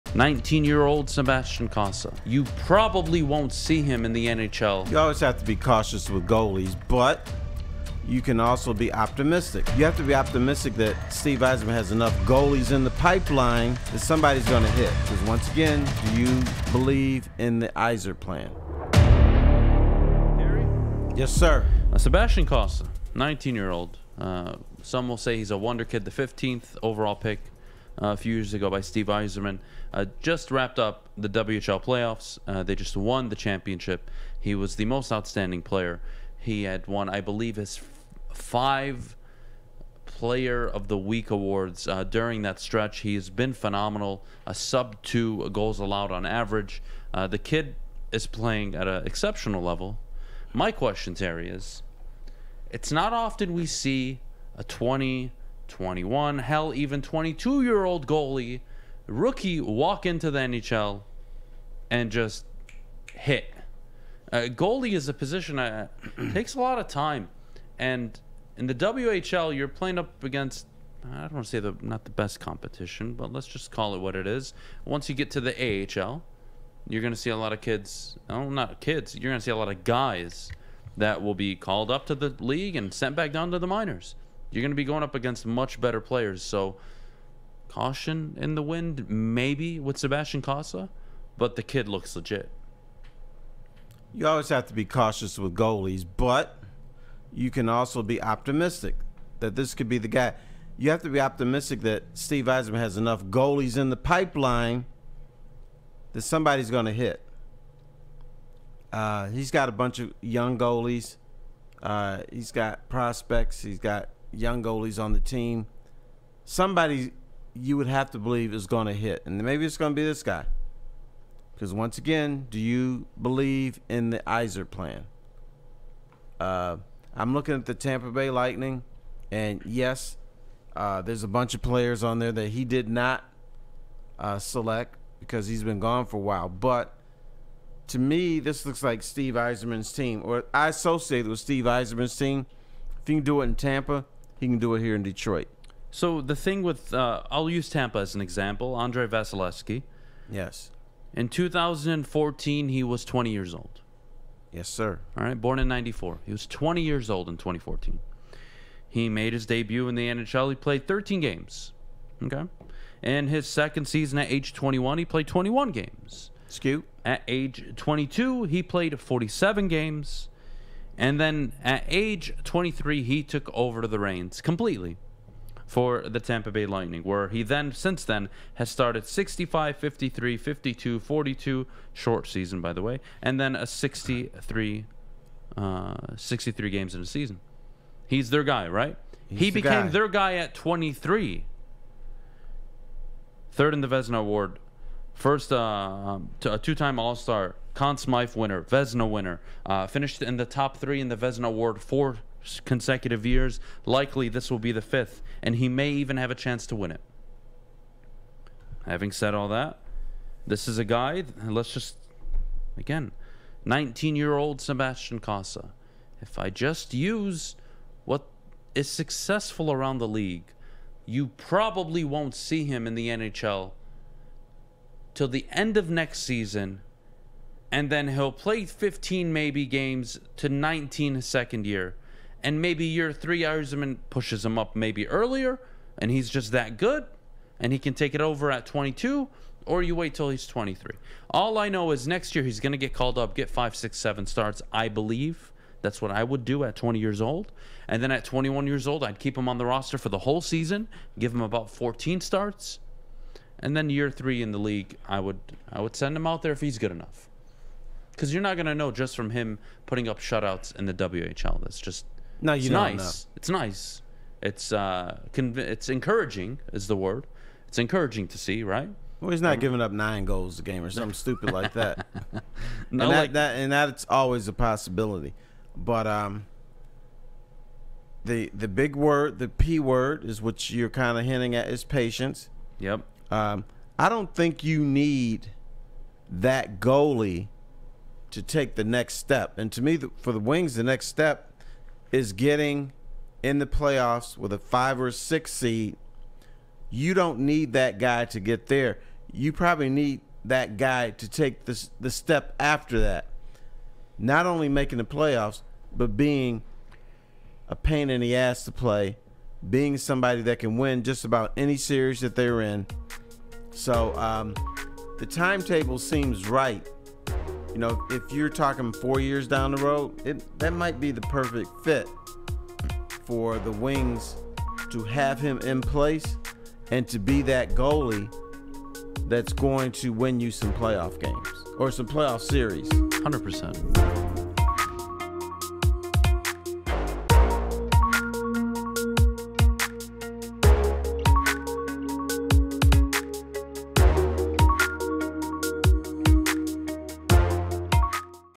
Sebastian Cossa is the FUTURE of the Detroit Red Wings
Adham Beydoun projects the timeline he expects Sebastian Cossa to follow during his rise through the Detroit Red Wings system. Cossa was one of the leading factors in why the Edmonton Oil Kings claimed the WHL Championship in six games, the final being a 2-0 shutout by Netminder Sebastian Cossa. Should Red Wing fans be excited?

The Bottom Line | Monday-Friday 2pm-4pm
Woodward Bets | Monday-Friday 6pm-7pm & Saturday-Sunday 9am-10am